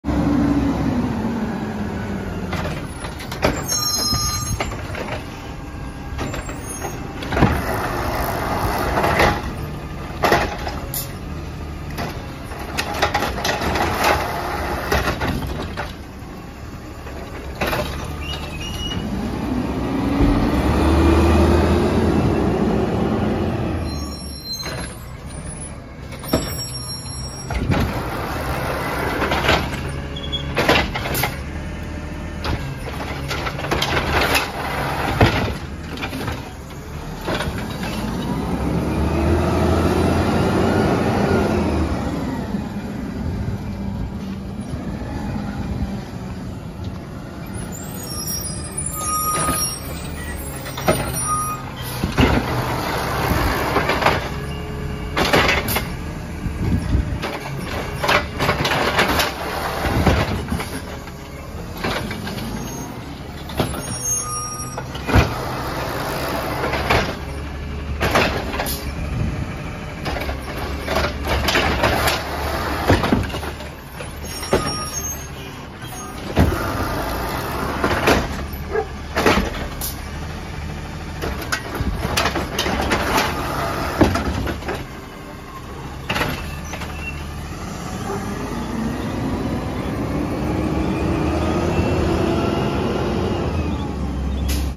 Calmet services trash truck #151 Peterbilt 320 part 88
Got to see this truck again and for doing my area in the other side